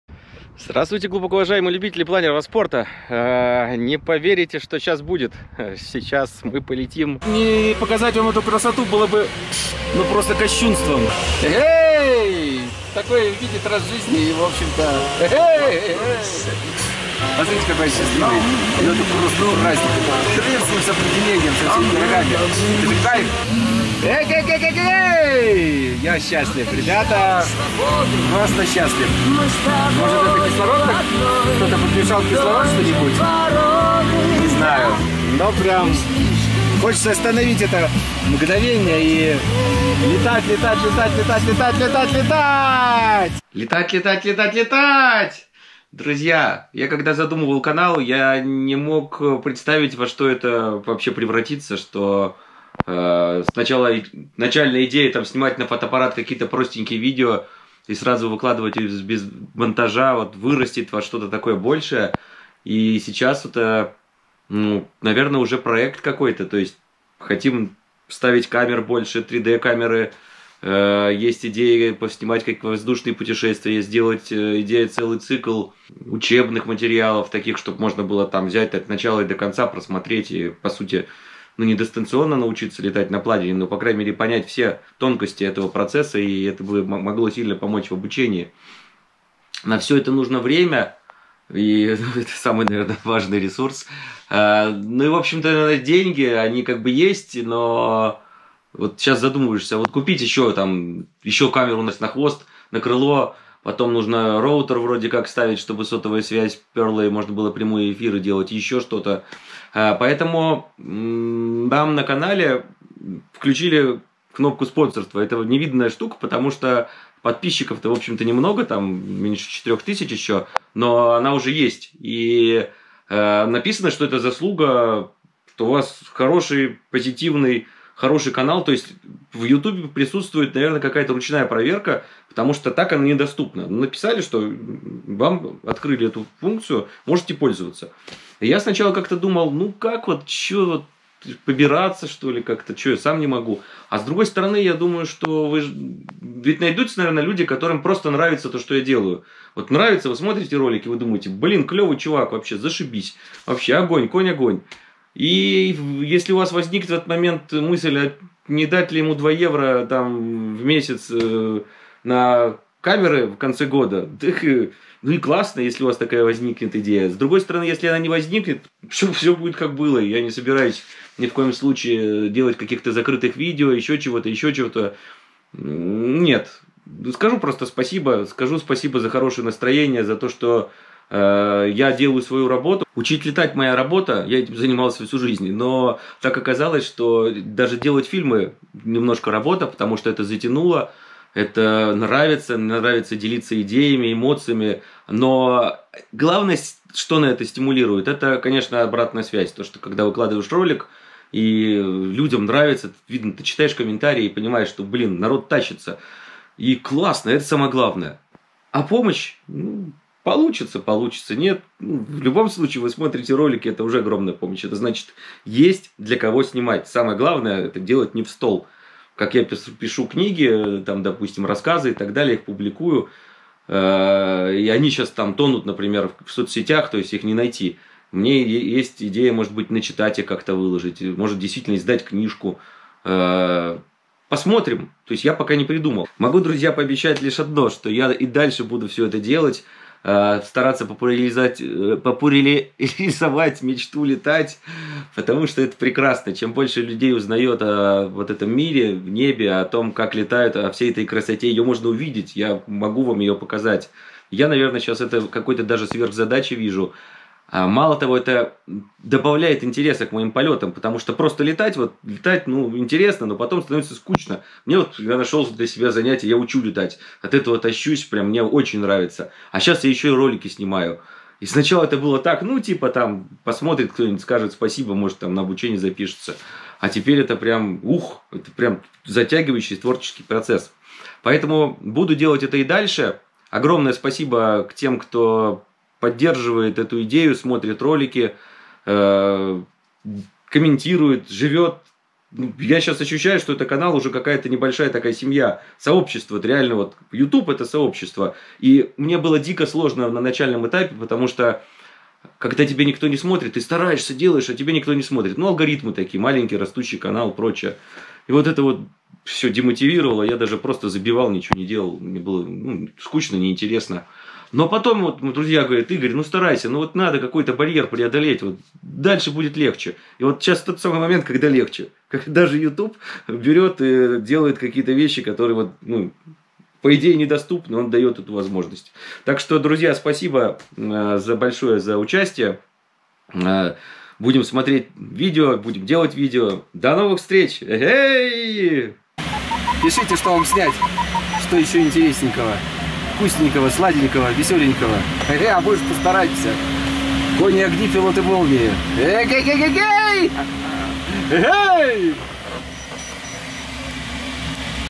Хорошие новости от YouTube. Неожиданно, каналу про планера и Мечту Летать выдали кнопку Спонсировать
Приятно когда  YouTube ценит твою работу. Я счастлив! Поэтому в очередной раз, здравствуйте глубокоуважаемые любители планерного спорта.
Изначальная идея канала, начатого в 2018 году, была простые, живые видеорепортажи с неба почти без монтажа. Снимал я их на фотоаппарат Сони и думать не мог, что этот проект перерастет в нечто большее.

Повезло с тем, что я очень многому научился, во многом именно и благодаря общению с Вами, замечательными людьми, неравнодушными к небу.

Сделано немало, но хочется еще больше!

Все эти планы требуют времени и средств. К счастью, и то и другое хоть и частично но имеется. Хоть и не скрою, что в наше не простое время я уже серьезно задумываюсь покупать ли дополнительную камеру или качественный накопитель для видео материалов.

Всю мою жизнь деньги были не целью, а всего лишь дополнительным элементом свободы. Свободы делать и творить именно так как хочешь. Максимально интересно, позитивно, качественно.
Именно поэтому все ресурсы с этого канала я отправляю на его улучшение. Новые камеры, новые ракурсы, новые места, красоты и бескрайние горизонты событий.

Я не знаю за какие заслуги наш скромный  (3500) канал получил эту привилегию. Написали - вы делаете интересный уникальный контент, при проверке было вынесено решение поддержать ваш канал.

Это чертовски приятно. Просто очень! Потому что я помню время два года назад, когда мои репортажи с неба смотрели пару десятков человек. Я старался, снимал, а результат был минимален. Это дико демотивировало. Пару раз бросал затею. Выручили друзья. Они постоянно твердили - Игорь, работай, все придет, нужно только время и старание. Еще чуть-чуть и станет легче.

И ведь стало. Появились благодарные люди, впервые узнавшие мир планеризма через мои видео. Это очень мотивировало и помогало творить. Помощь друга Сергея вывела качество видео на новый уровень, да и я наконец то научился монтировать.

Канал пошел вверх, и теперь, я уже на 100 процентов уверен в успехе.
Люди не равнодушны к Мечте Летать. А значит нам есть для кого снимать и творить.

Мне всегда нравилось работать в Команде. И сейчас вы - моя команда. Люди, неравнодушные к небу.

Если Вам интересен и полезен контент на канале и у Вас есть возможность оценить нашу работу, то будут доступны три варианта участия.

Бриз - спокойное и уверенное движение к цели.

Мистраль - мощный поток рождающий атмосферные волны с бесконечной свободой высоты.

Джет стрим - сверхскоростные атмосферные течения способные унеси планер в приключение кругосветного путешествия.

Самая же главная помощь - Ваши эмоции. Именно они помогают творить радость в небе и нести красоту полетов самые разные  уголки мира.
Будем Летать!

Буду признателен за разнообразные отзывы, добрые советы и конструктивную критику.

Ваш небесный странник
Игорь Волков.